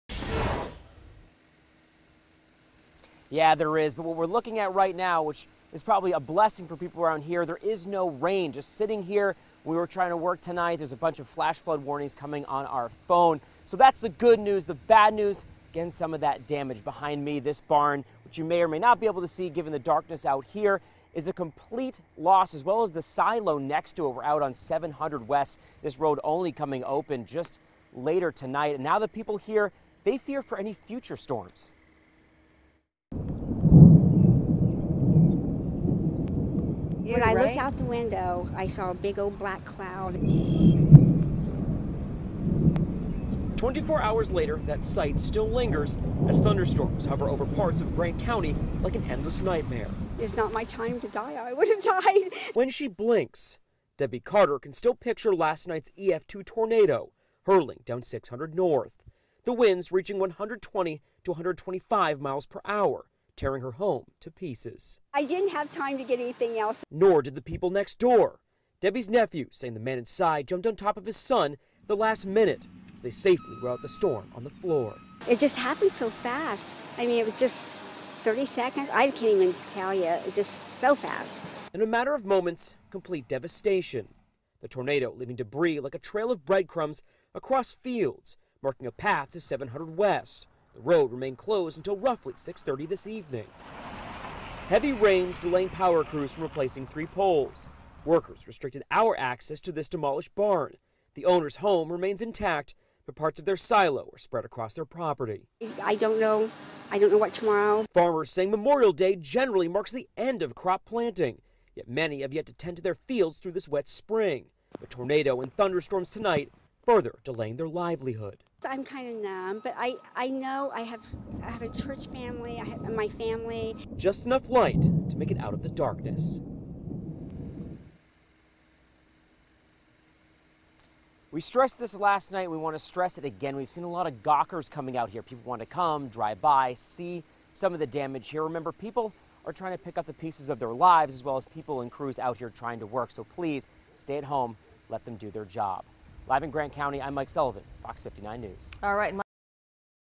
Tornado moves through Grant County
A look at the considerable tornado damage in Grant County, Indiana.